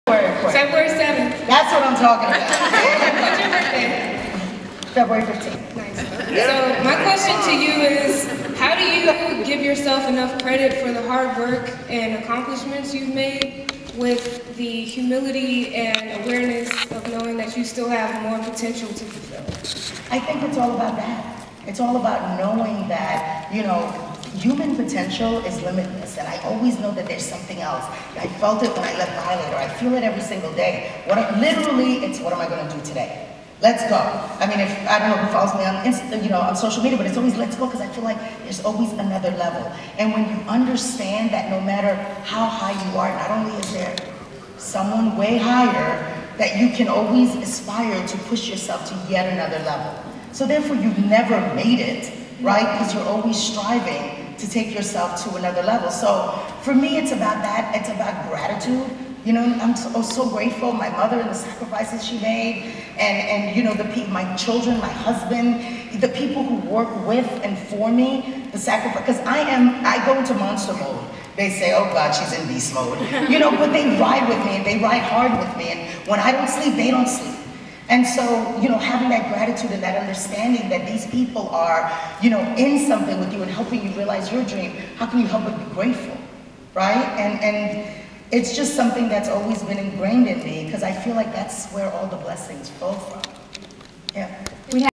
Mona Scott-Young Discusses How to Go to Another Level After Achieving Success
Television producer Mona Scott-Young on how to go to the next level after completing a goal during a panel at the 2017 American Black Film Festival in Miami.